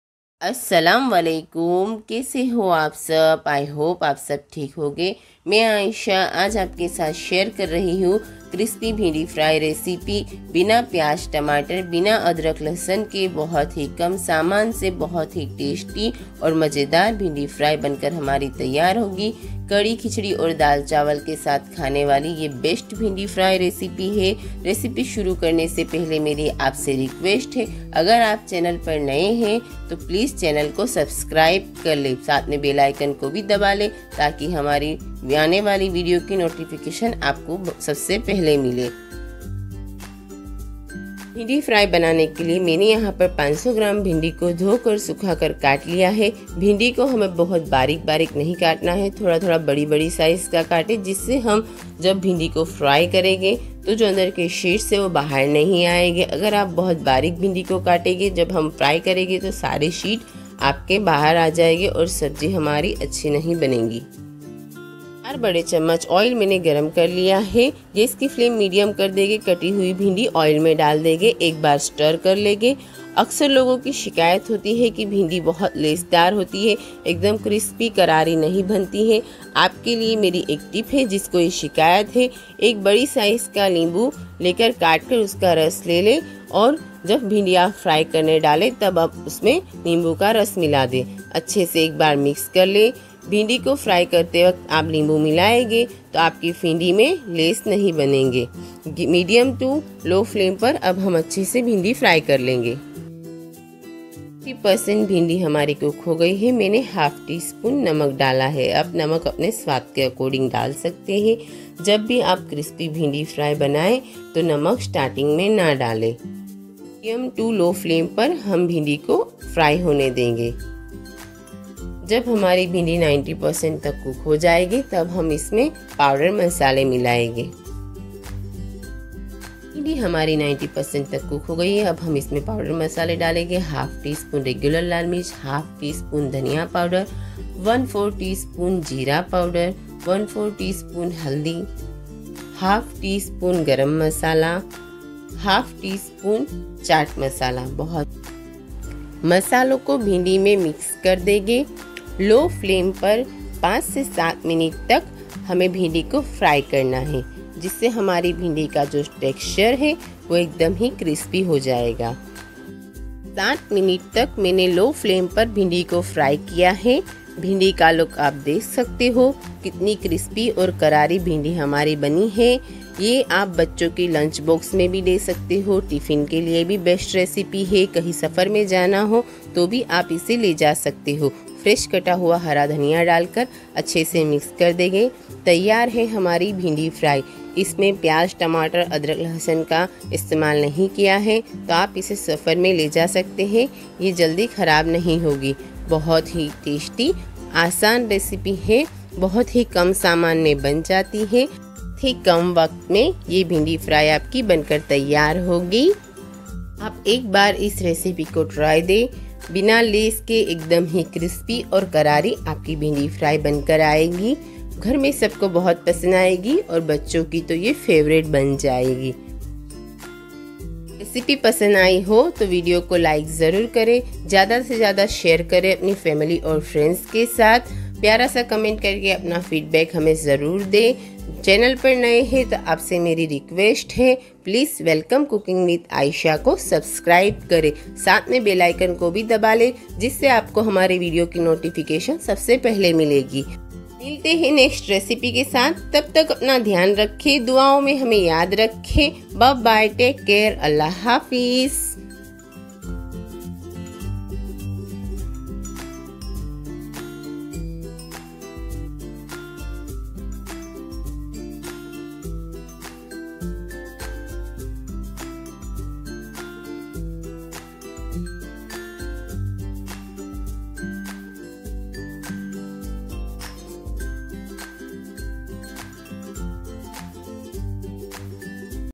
Bhindi Fry | बच्चो के टिफिन ओर सफर के लिए Best Crispy Bhindi Fry Recipe
Hi everyone Aaj mene aapke sath share ki Crispy Bhindi Fry ki recipe lunchbox or safar ke liye best recipe try kare.